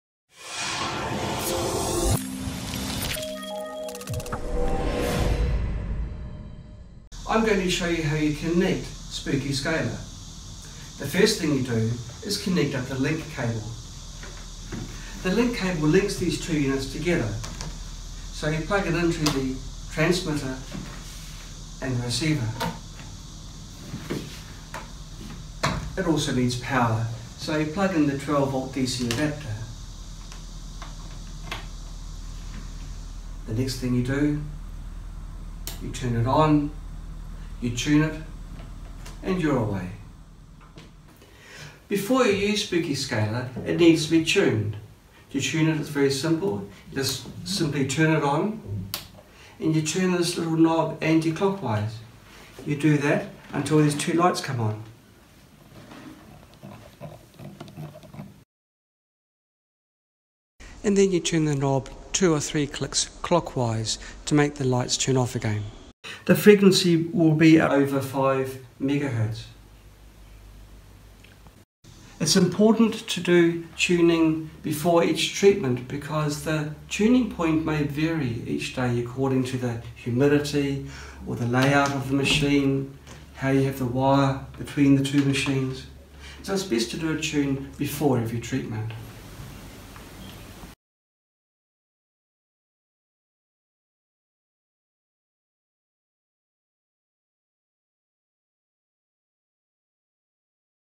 How to Set up and Tune Spooky2 Scalar
Just like a radio, Spooky Scalar requires tuning. It is very simple to do.

Place the Tune PCB on the Transmitter “Tuning PCB Position”. Turn the Tuning Adjustment knob anticlockwise until the lights on the PCB start to turn on. Then turn the knob 2 click positions clockwise so the lights are off. Remove the Tune PCB after tuning.